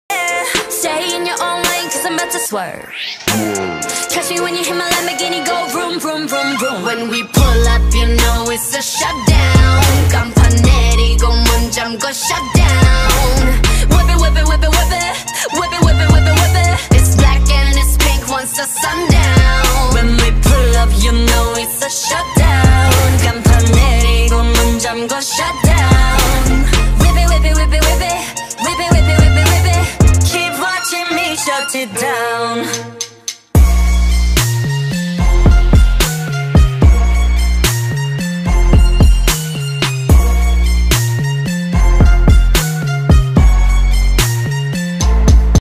Finally Collected full set of BlackPink orea photocard BlackPink 【GreenRiceBall】
it is easy to 100% choose what you wan't.